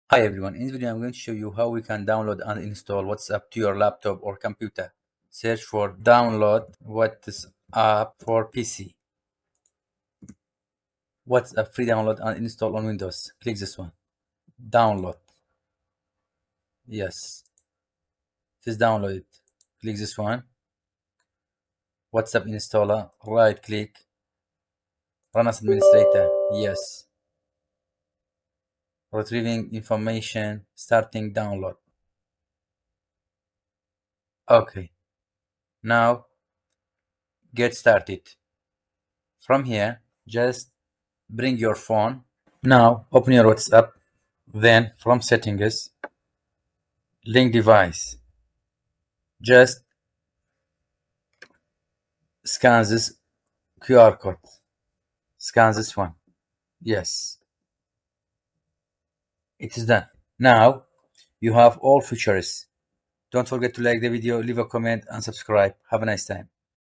How to Download and Install WhatsApp in Laptop or PC
In this video, we’ll show you **how to download and install WhatsApp** on your **laptop** or **PC** with easy-to-follow steps! Whether you're using **Windows** or **Mac**, we’ve got you covered with all the necessary steps to get WhatsApp up and running on your desktop.

We’ll guide you through the process of:
- **Downloading WhatsApp for PC or laptop** from the official website.
- **Installing WhatsApp** on **Windows** and **Mac OS**.
- Setting up **WhatsApp Web** and syncing it with your phone for seamless communication.
- Troubleshooting common issues and ensuring that WhatsApp works smoothly on your laptop or PC.

By the end of this tutorial, you’ll be able to enjoy the full WhatsApp experience right on your desktop, making it easier to manage your chats and stay connected with friends, family, and colleagues.